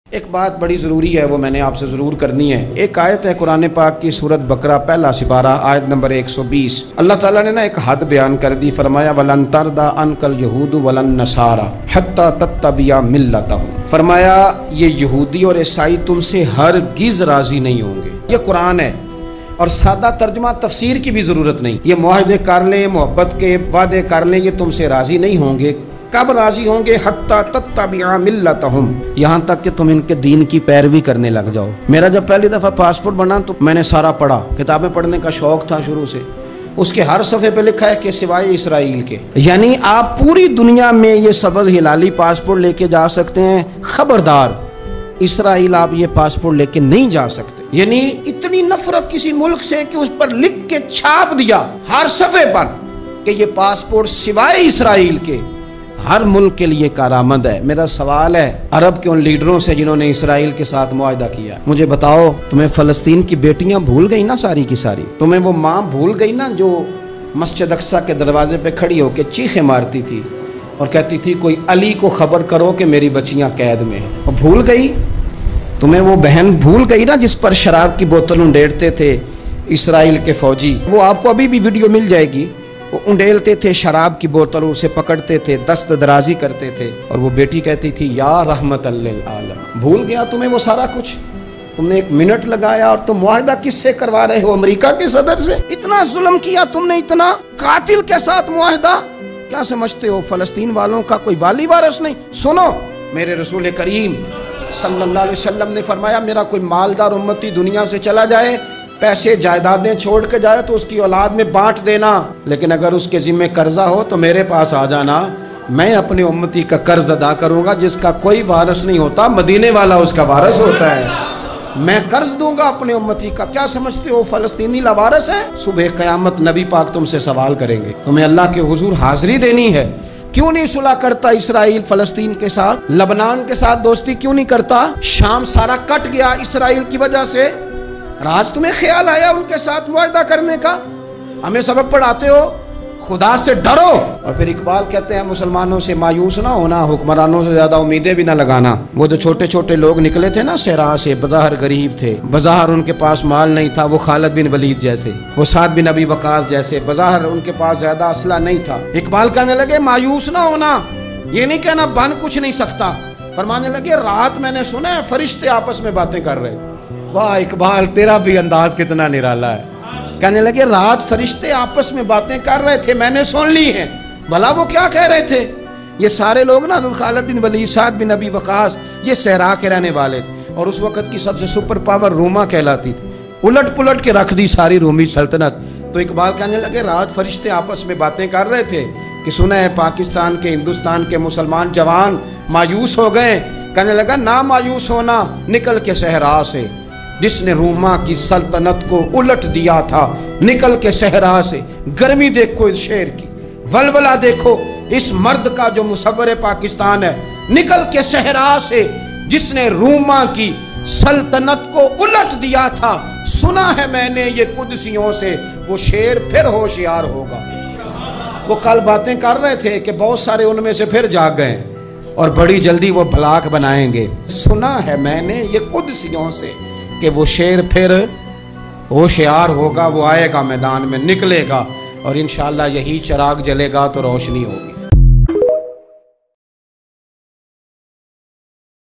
Peer Ajmal Raza Qadri || New Letest Bayn About Phalstine || Must watch
Peer Ajmal Raza Qadri Ne Haqeeqat Bayn ki
mashallah khud b sune dosto ky sath family ky sath b share karain shukriya